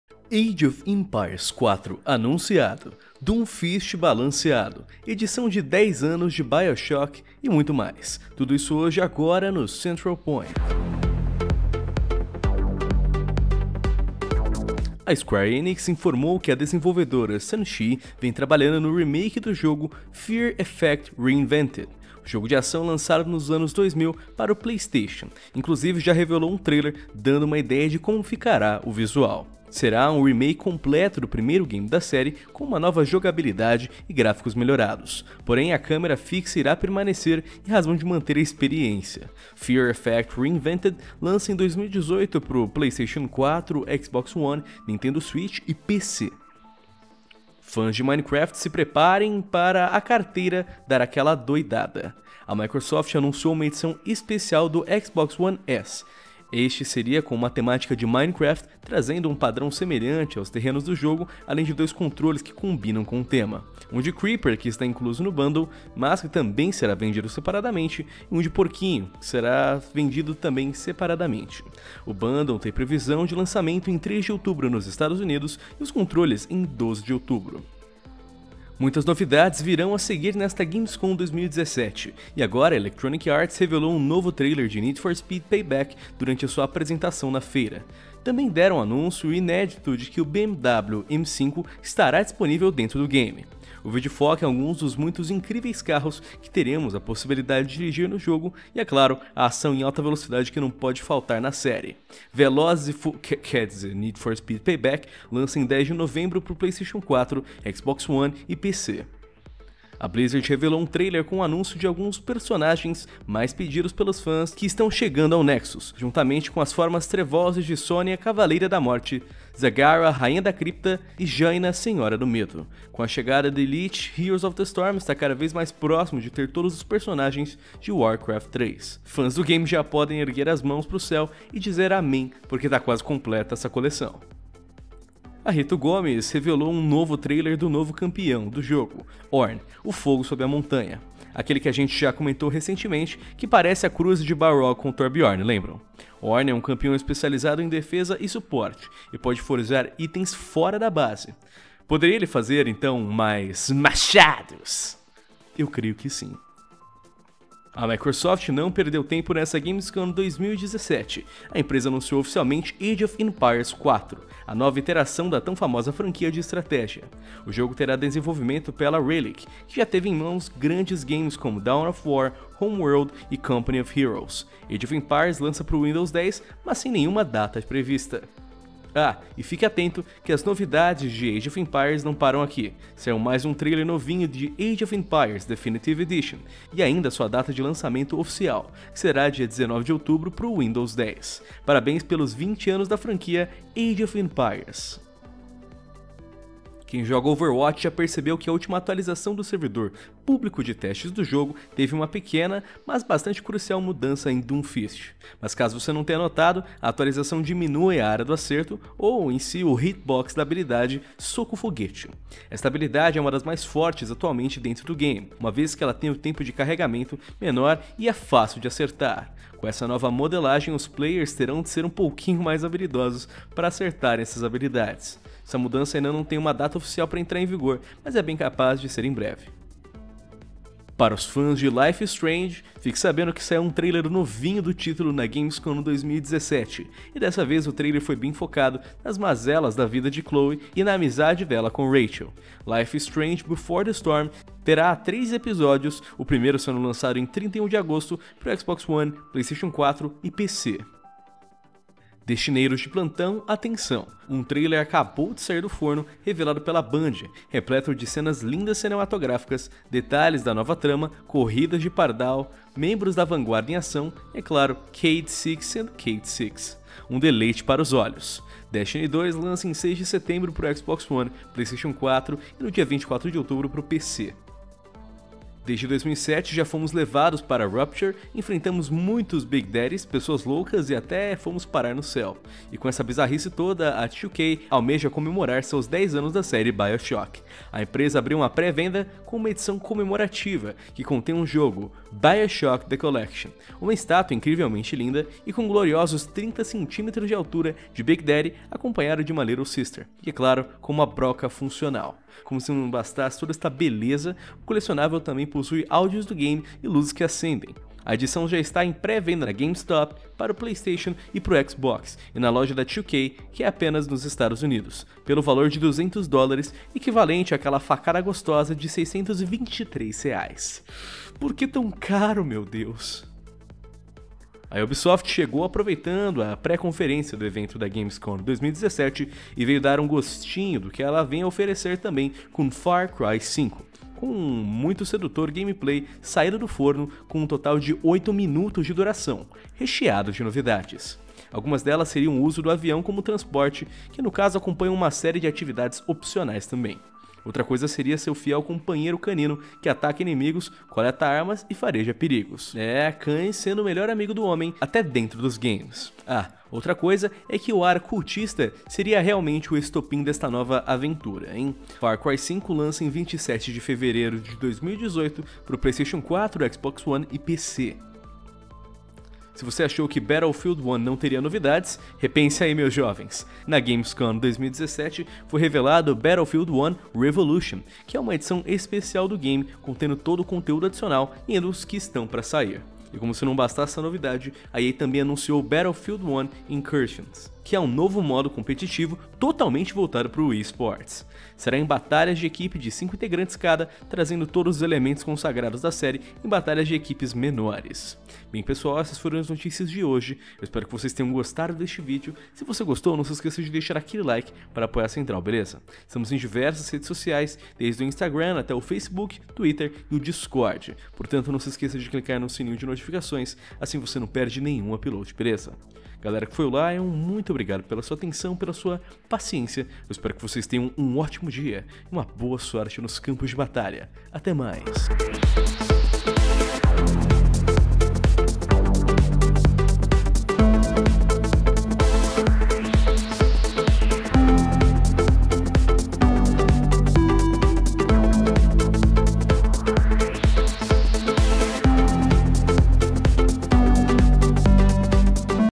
AGE OF EMPIRES 4 ANUNCIADO, DOOMFIST NERFADO E MUITO MAIS! - CENTRAL POINT
A Gamescom começou, e agora é hora de conferir todas as últimas novidades anunciadas no maior evento gamer da Europa.